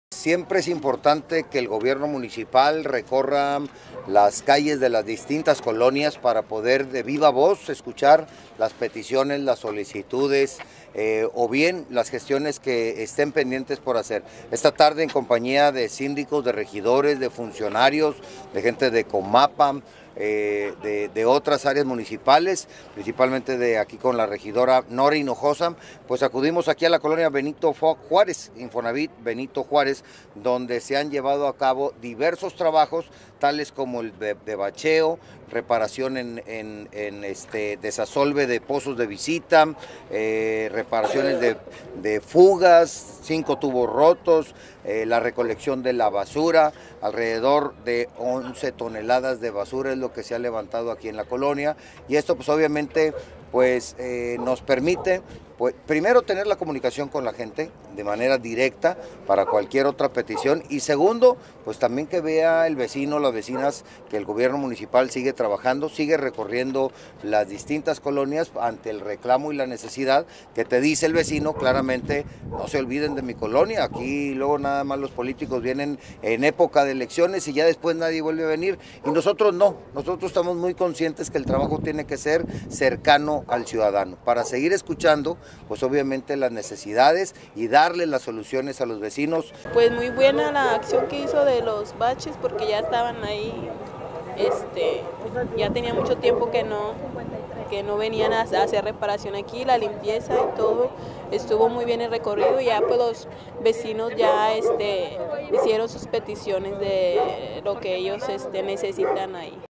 Atiende Rivas con PAI colonia Benito Juárez
Con el Programa Acción Inmediata (PAI), el presidente municipal Enrique Rivas recorrió calles de la colonia Infonavit Benito Juárez, escuchó y atendió peticiones de bacheo, reparación de alumbrado, limpieza y asesorías jurídicas que le hicieron las y los vecinos del sector.